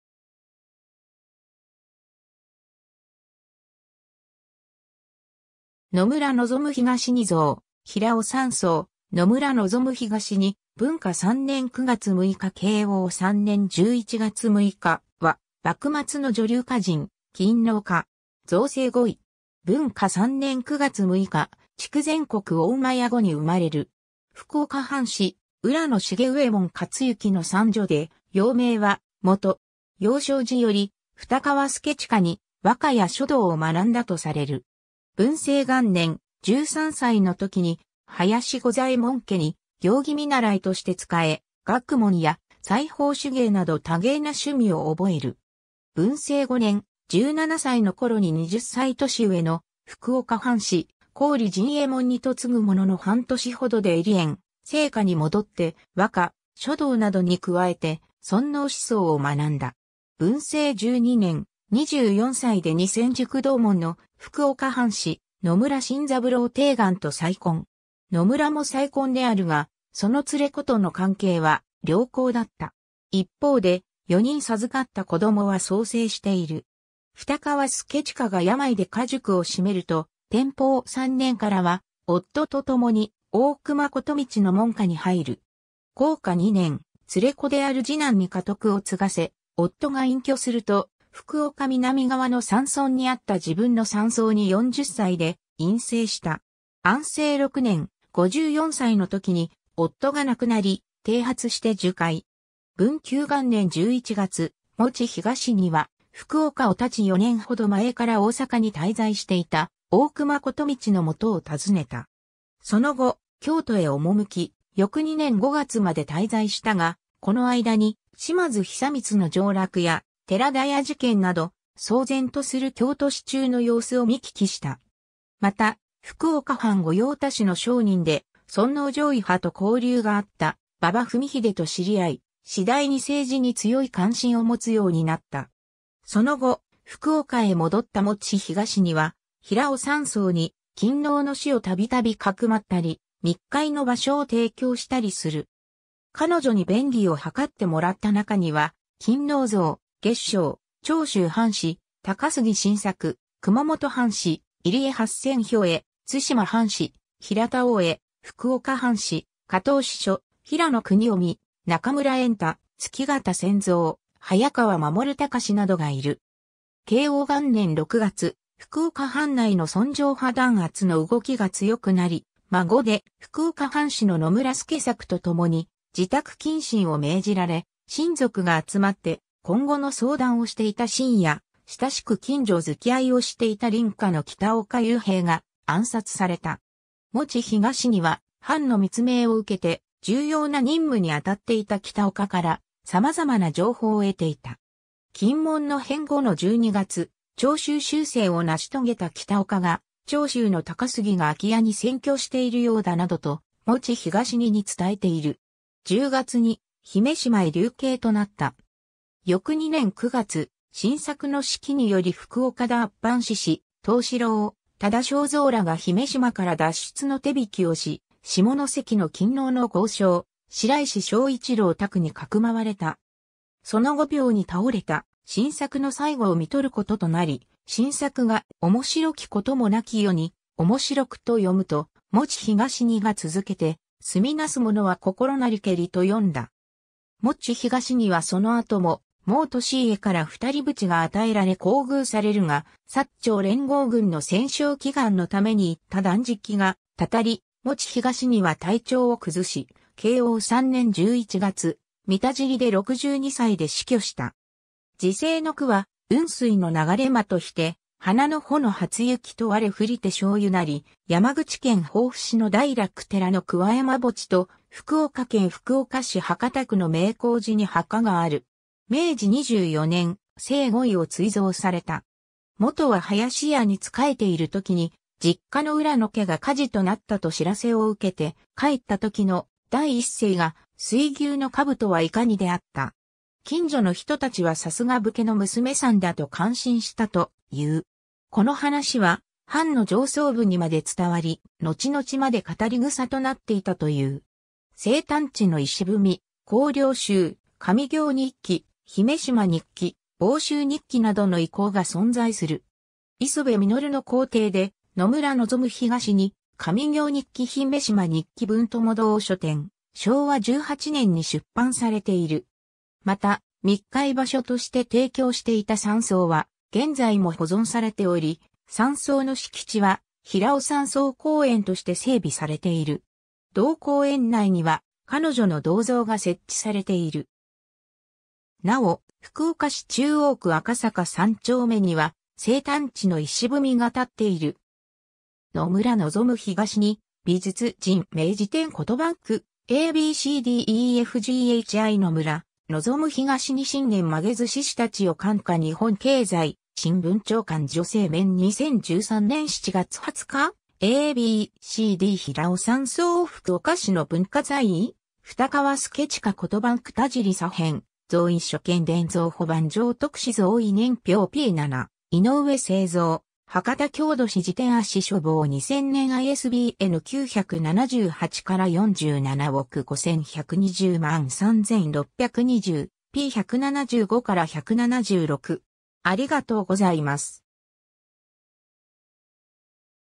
野村望東尼
野村望東尼像 平尾山荘 野村 望東尼（のむら もとに、ぼうとうに）、文化3年9月6日（1806年10月17日） - 慶応3年11月6日（1867年12月1日））は、幕末の女流歌人・勤王家。
贈正五位。
 文化3年（1806年）9月6日、筑前国御厩後（現福岡県福岡市中央区赤坂）に生まれる。
福岡藩士・浦野重右衛門勝幸の三女で、幼名は"モト"。
幼少時より二川相近に和歌や書道を学んだとされる。
 文政元年（1818年）、13歳の時に林五左衛門家に行儀見習いとして仕え、学門や裁縫手芸など多芸な趣味を覚える。
 文政5年（1822年）、17歳の頃に20歳年上の福岡藩士郡甚右衛門に嫁ぐものの半年ほどで離縁、生家に戻って和歌・書道などに加えて尊皇思想を学んだ。
 文政12年（1829年）、24歳で二川塾同門の福岡藩士・野村新三郎貞貫と再婚。
野村も再婚であるが、その連れ子との関係は良好だった。
一方で、4人授かった子どもは早世している。
 二川相近が病で家塾を閉めると、天保3年（1832年）からは夫と共に大隈言道の門下に入る。
 弘化2年（1845年）、連れ子である次男に家督を継がせ夫が隠居すると、福岡南側の山村 （現・福岡市中央区平尾）にあった自分の山荘（平尾山荘）に40歳で隠棲した。
 安政6年（1859年）、54歳の時に夫が亡くなり、剃髪して受戒。
 文久元年（1861年）11月、望東尼は福岡を発ち4年ほど前から大坂に滞在していた大隈言道の元を訪ねた。
 その後、京都へ赴き、翌2年5月まで滞在したが、この間に島津久光の上洛や寺田屋事件など、騒然とする京都市中の様子を見聞きした。
また、福岡藩御用達の商人で尊皇攘夷派と交流があった馬場文英と知り合い、次第に政治に強い感心を持つようになった。
その後、福岡へ戻った望東尼は平尾山荘に勤皇の士を度々かくまったり、密会の場所を提供したりする。
彼女に便宜を図って貰った中には、勤王僧・月照、長州藩士・高杉晋作、熊本藩士・入江八千兵衛、対馬藩士・平田大江、福岡藩士・加藤司書、平野国臣、中村円太、月形洗蔵、早川養敬などがいる。
 慶応元年（1865年）6月、福岡藩内の尊攘派弾圧の動きが強くなり、孫で福岡藩士の野村助作（夫と前妻の孫）と共に自宅謹慎を命じられ、親族が集まって今後の相談をしていた深夜、親しく近所づきあいをしていた隣家の喜多岡勇平が暗殺された。
望東尼は藩の密命を受けて重要な任務にあたっていた喜多岡から様々な情報を得ていた。
禁門の変後の12月、長州周旋を成し遂げた喜多岡が「長州の高杉が空き家に潜居しているようだ」などと望東尼に伝えている。
 10月に姫島（現・福岡県糸島市志摩姫島）へ流刑となった。
(乙丑の獄) 翌2年（1866年）9月、晋作の指揮により福岡脱藩志士・藤四郎、多田荘蔵らが姫島から脱出の手引きをし、下関の勤皇の豪商・白石正一郎宅に匿われた。
その後病に倒れた晋作の最期を看取る事となり、晋作が「おもしろき 事もなき世に おもしろく」と詠むと、望東尼が続けて「住みなすものは 心なりけり」と詠んだ。
 望東尼はその後も毛利家から二人扶持が与えられ厚遇されるが、薩長連合軍の戦勝祈願のために行った断食が祟り 、望東尼は体調を崩し、慶応3年（1867年）11月、三田尻（現・山口県防府市の古称）で62歳で死去した。
 辞世の句は「雲水の ながれまとひて花の穂の 初雪とわれふりて消ゆなり」 山口県防府市の大楽寺の桑山墓地と福岡県福岡市博多区の明光寺に墓がある。
 明治24年（1891年）、正五位を追贈された。
 モトは林家に仕えている時に実家の浦野家が火事となったと知らせを受けて、帰った時の第一声が「水牛の兜はいかに」であった。
近所の人達はさすが武家の娘さんだと感心したという。
この話は藩の上層部にまで伝わり、後々まで語り種となっていたという。
 生誕地の碑 「向陵集」「上京日記」「姫島日記」「防州日記」などの遺稿が存在する。
磯辺実の校訂で、野村望東尼『上京日記 姫島日記』文友堂書店、昭和18年（1943年）に出版されている。
 また、密会場所として提供していた山荘は現在も保存されており、山荘の敷地は平尾山荘公園として整備されている。
同公園内には彼女の銅像が設置されている。
なお、福岡市中央区赤坂3丁目には生誕地の碑が立っている。
 ^ 野村望東尼 - 美術...